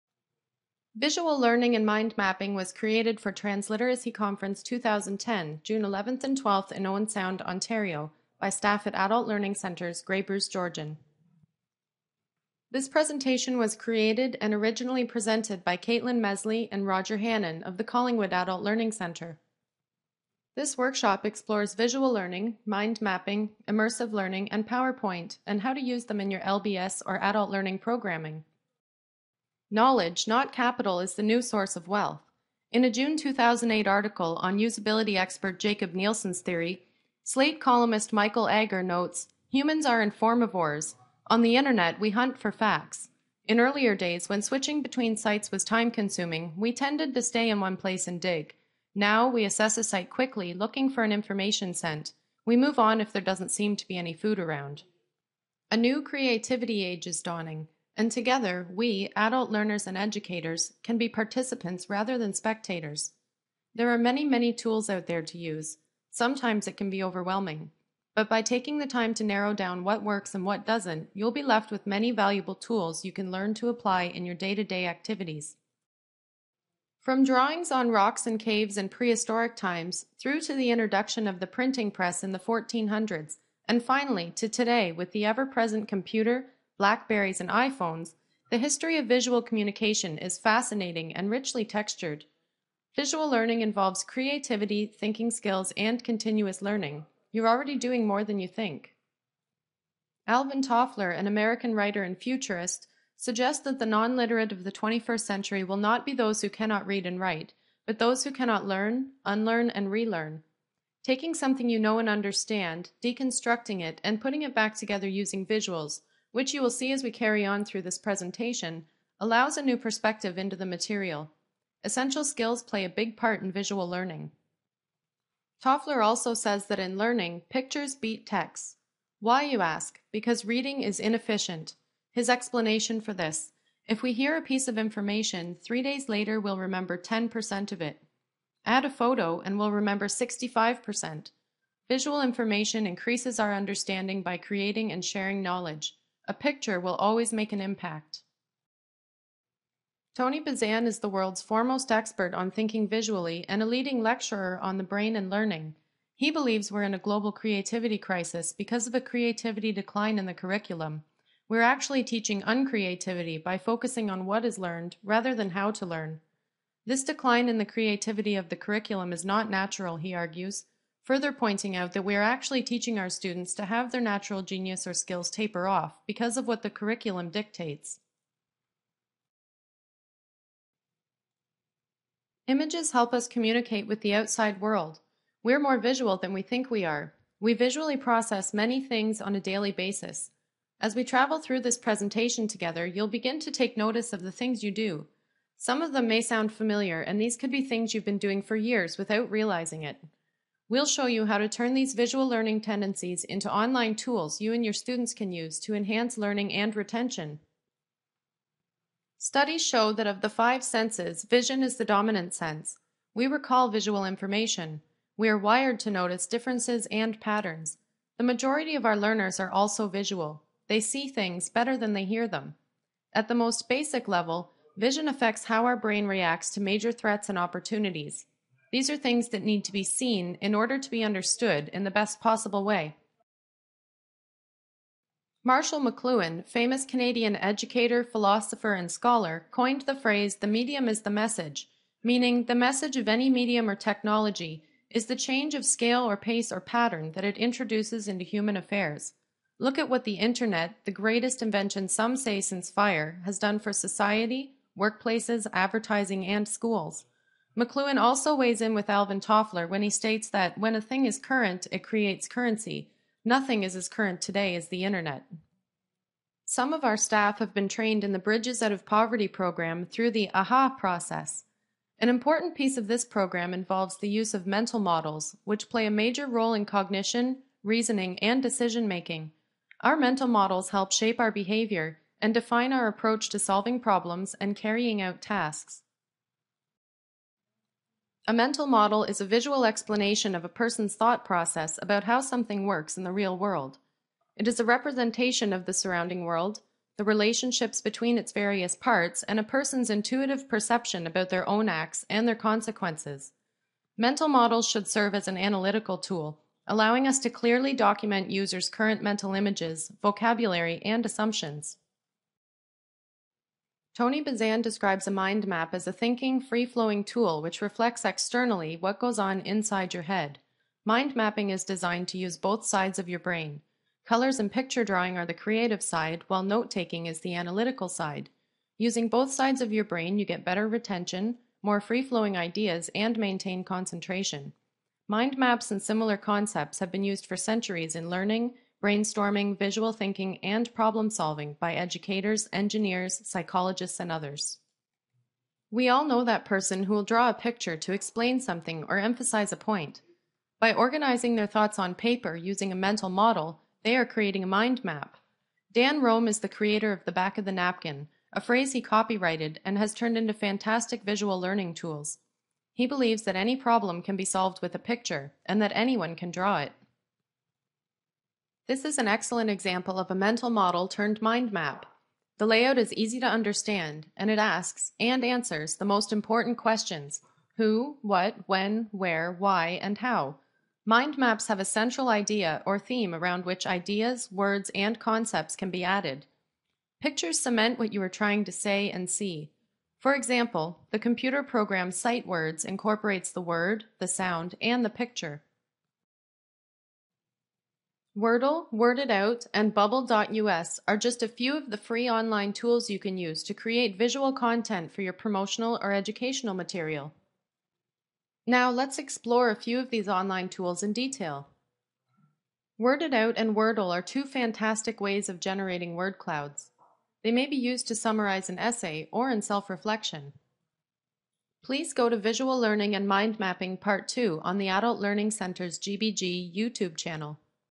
Visual Learning and Mind Mapping Part 1 - Transliteracy Conference 2010
Part 1 of an e-learning resource for LBS and adult learning practitioners. Visual Learning & Mind Mapping was created and originally presented by Roger Hannon and Kaitlyn Mesley of Adult Learning Centres Grey-Bruce-Georgian for Transliteracy Conference 2010 in Owen Sound, Ontario.